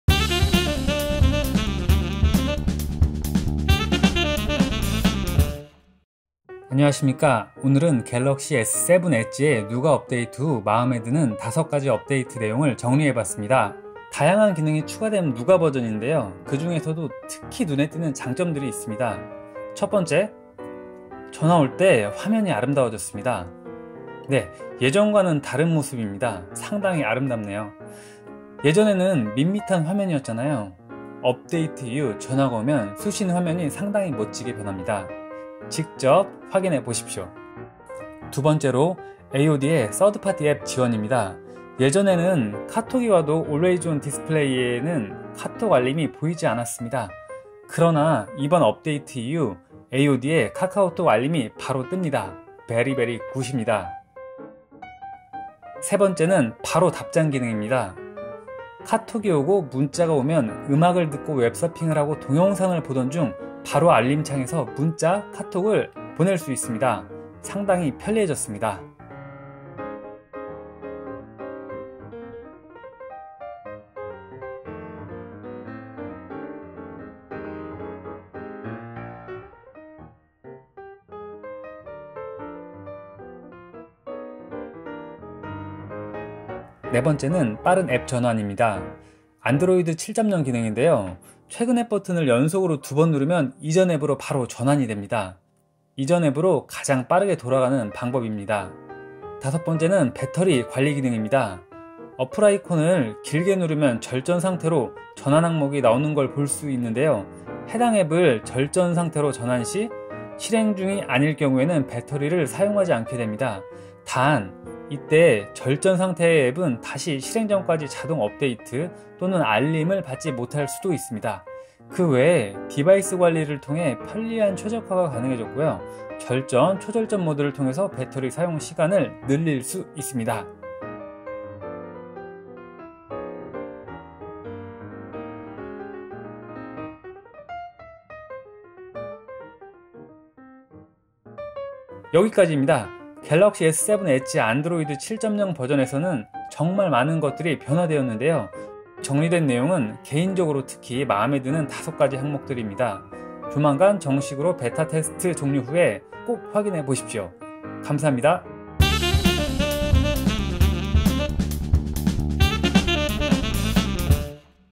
안드로이드 7.0 '누가' 탑재된 갤럭시s7 엣지 이런 기능 맘에 든다!(galaxy s7 edge android 7.0 nougat)
현재 베타펌으로 갤럭시s7 엣지의 안드로이드 7.0 누가 버전을 사용하고 있는데요. 개인적으로 맘에 드는 몇가지 기능들을 정리해 봤습니다.(galaxy s7 edge android 7.0 nougat)